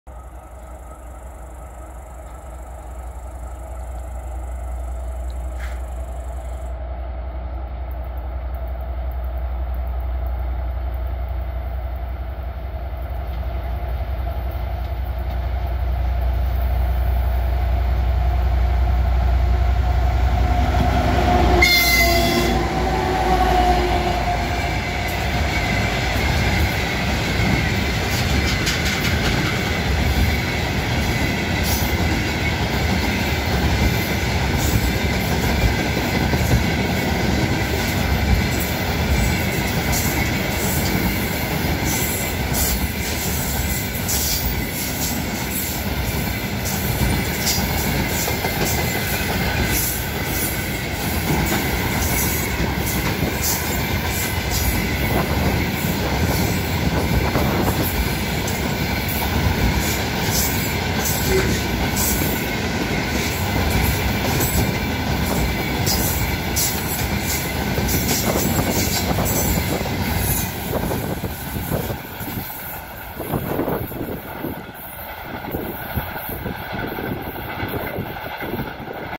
2ТЭ10УК-0169 с контейнерным поездом.
Тепловоз 2ТЭ10УК-0169(Депо Гомель) с грузовым поездом следовал по перегону Могилёв-1-Лотва.Линия Могилёв-Орша.07.09.2024 18:04